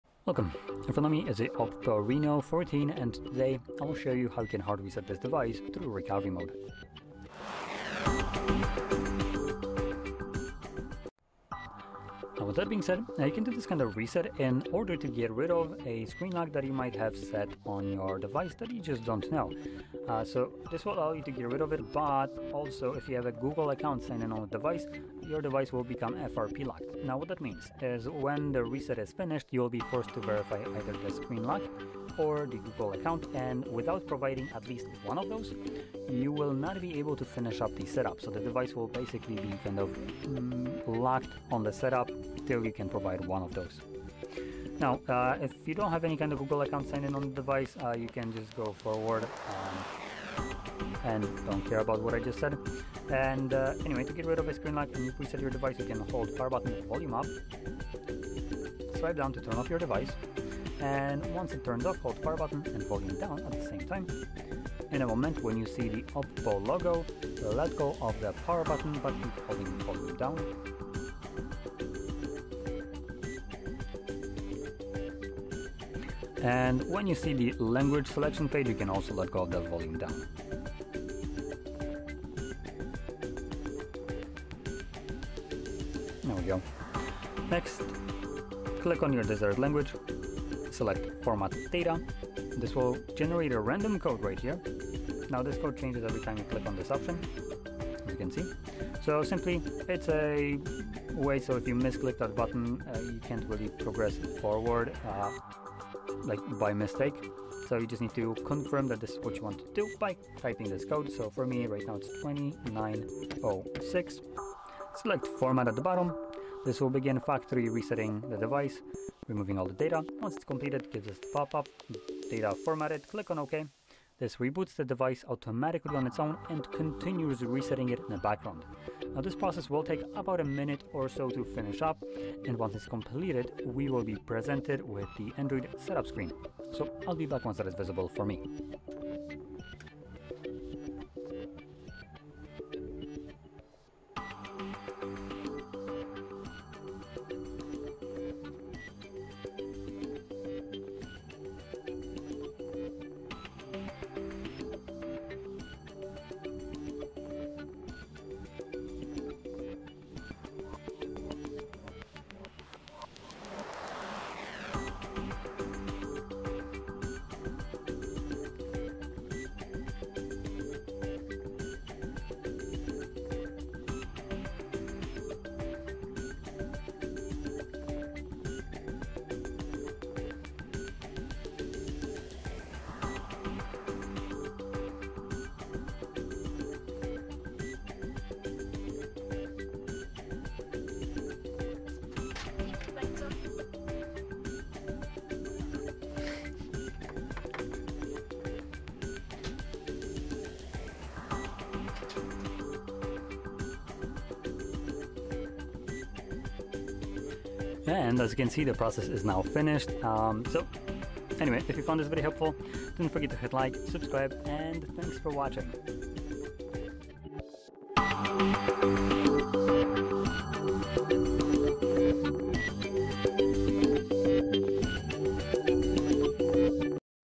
OPPO Reno 14 – How to Hard Reset with Recovery Mode
If your OPPO Reno 14 is experiencing issues like screen lock, lag, or you simply want to erase all data and start fresh, performing a hard reset through recovery mode can help. In this video, we show you step-by-step how to hard reset the OPPO Reno 14 using the hardware buttons and recovery menu. This process will remove all personal data, apps, and settings from your device, returning it to factory defaults. Please note, if your device has a Google account signed in, you will need to verify your account or screen lock after the reset due to FRP (Factory Reset Protection). Follow along to safely reset your OPPO Reno 14 and resolve common problems quickly and easily.

How to hard reset OPPO Reno 14 using recovery mode?
How to remove screen lock on OPPO Reno 14 with a hard reset?
What happens after a factory reset on OPPO Reno 14?

0:00 Introduction
0:11 Important information about Google account and FRP
0:44 Power off OPPO Reno 14
1:00 Enter recovery mode (button combination)
1:11 Select language and format data
1:35 Confirm reset with code
1:56 Start factory reset process
2:09 Device reboot and reset completion
2:28 Setup after reset
3:28 Outro